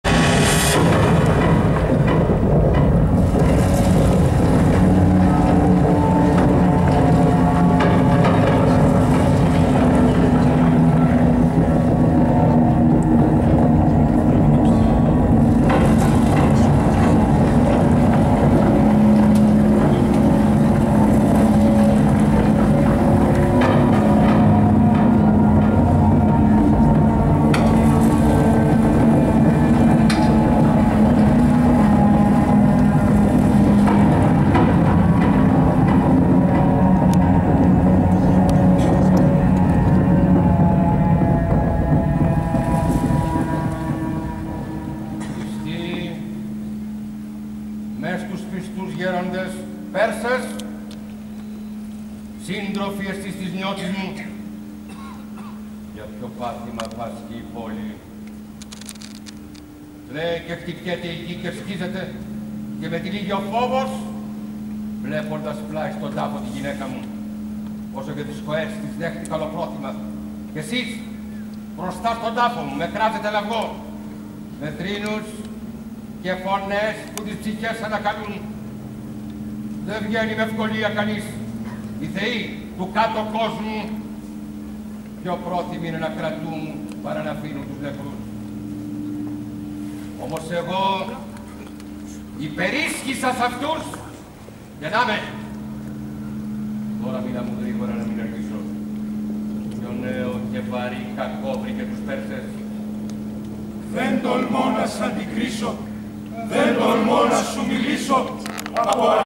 ΠΕΡΣΕΣ ΑΙΣΧΥΛΟΥ (Αισχύλεια 2014 28-08-2014)
Παρουσίαση της τραγωδίας Πέρσες του Αισχύλου από το Κρατικό Θέατρο Βορείου Ελλάδας στο Παλαιό Ελαιουργείο Ελευσίνας στα πλαίσια του Φεστιβάλ των Αισχυλείων το 2014.